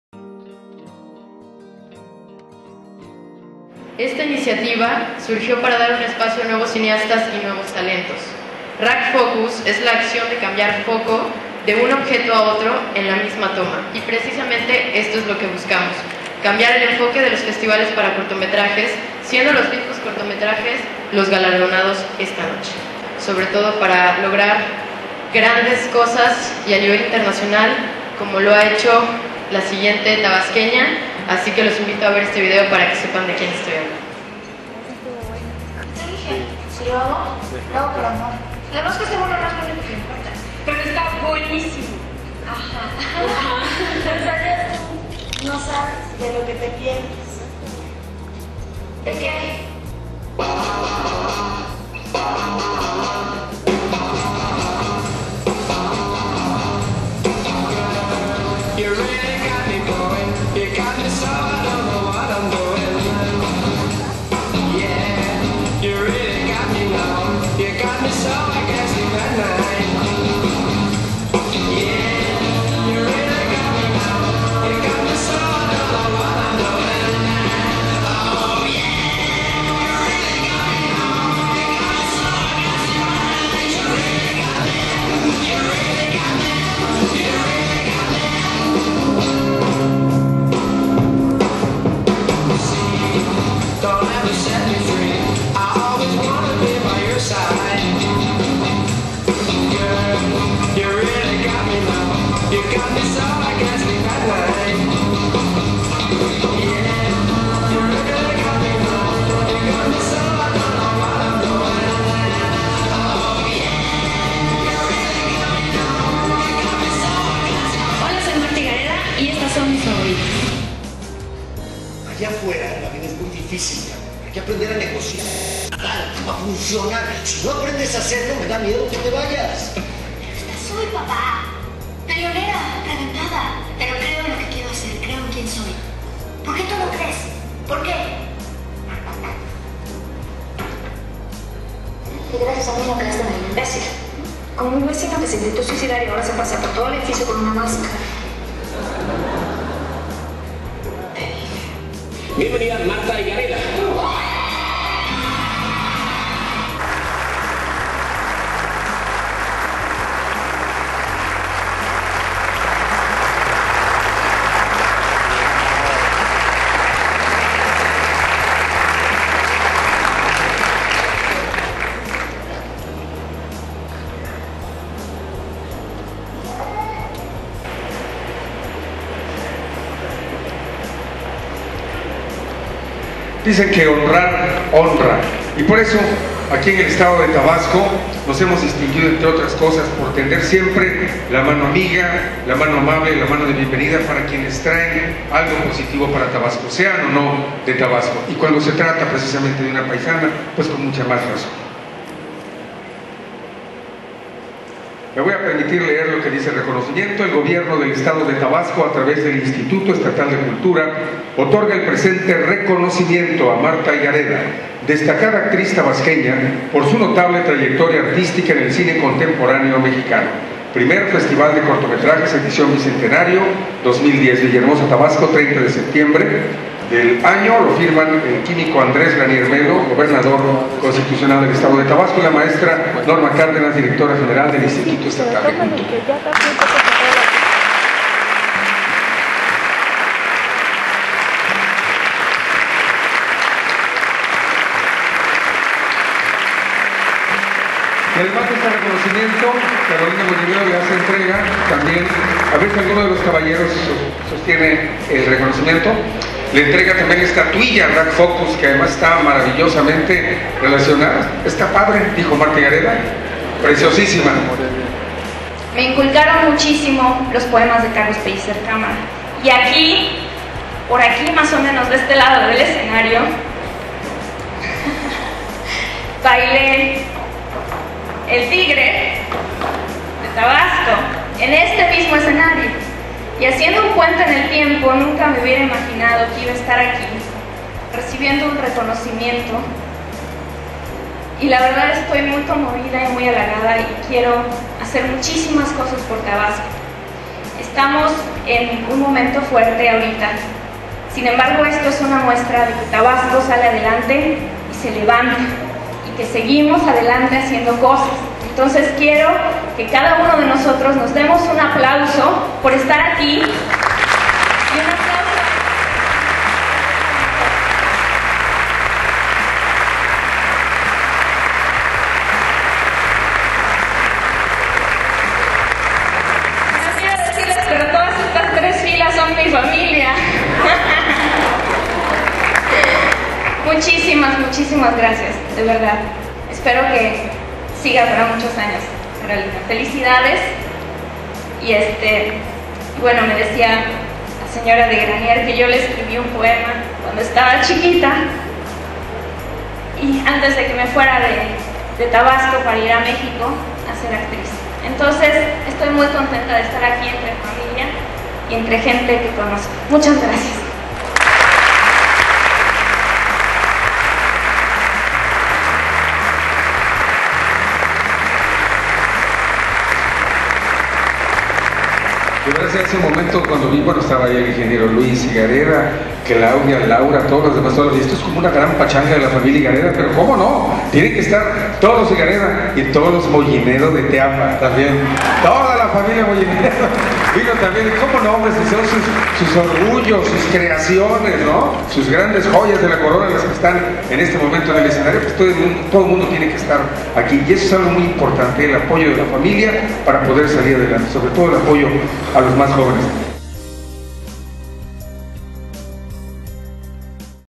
Homenaje a Martha Higareda.wmv
En el marco de la inauguración del 1er Festival de cortometrajes realizado en villahermosa Tabasco, la actriz Martha Higareda fue reconocida por su trayectoria artistica